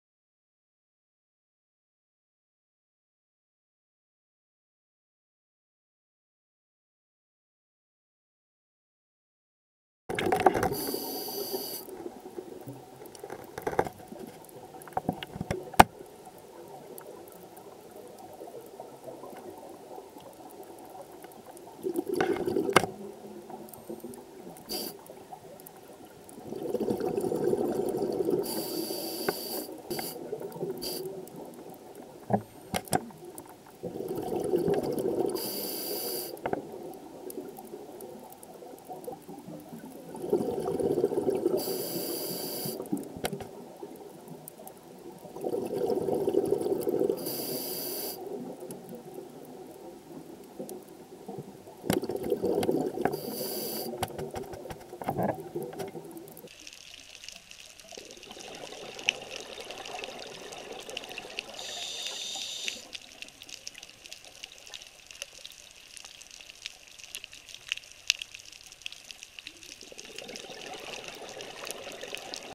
Pictichromis cf. diadema, yellow dottyback
Probably an all-yellow (xanthomorphic) variety of Pictichromis diadema.  Subtitle:  why it's impossible to shoot video with a dSLR of little moving fish (and stay in focus)!

[taxonomy:binomial=Pictichromis diadema]